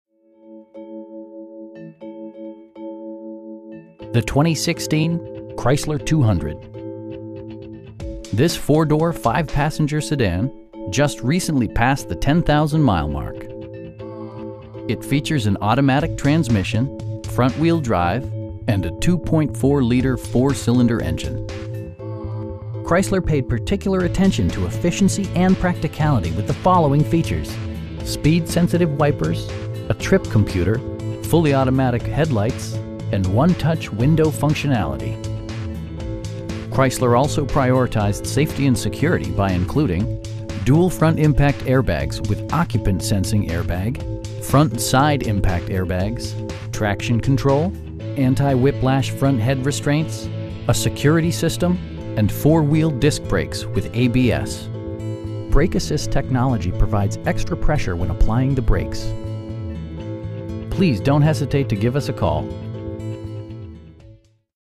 2016 Chrysler 200 Limited in Chesapeake, VA 23320
The 2016 Chrysler 200. This 4 door, 5 passenger sedan just recently passed the 10,000 mile mark! It features an automatic transmission, front-wheel drive, and a 2.4 liter 4 cylinder engine. Chrysler paid particular attention to efficiency and practicality with the following features: speed sensitive wipers, a trip computer, fully automatic headlights, telescoping steering wheel, cruise control, an overhead console, and 1-touch window functionality. Chrysler also prioritized safety and security by including: dual front impact airbags with occupant sensing airbag, front side impact airbags, traction control, anti-whiplash front head restraints, a security system, and 4 wheel disc brakes with ABS. Brake assist technology provides extra pressure when applying the brakes. Our aim is to provide our customers with the best prices and service at all times. Please don't hesitate to give us a call.